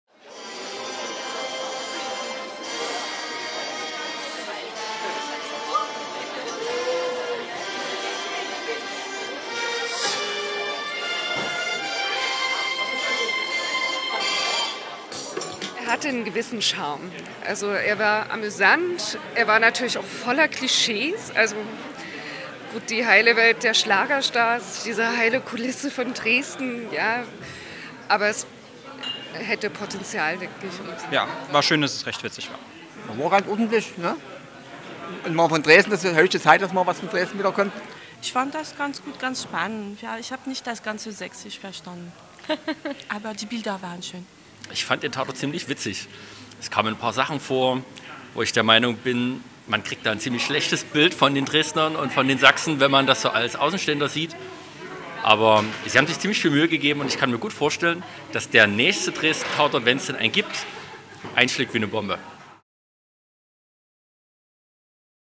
So fanden Dresdner den neuen Dresden-Tatort
Nach vielen Jahren ohne "Tatort" war es am Sonntag soweit: Der neue TV-Krimi aus Dresden wurde ausgestrahlt. Wir haben die Zuschauer in der Neustädter Scheune nach ihrer Meinung zum Dresden-"Tatort" gefragt.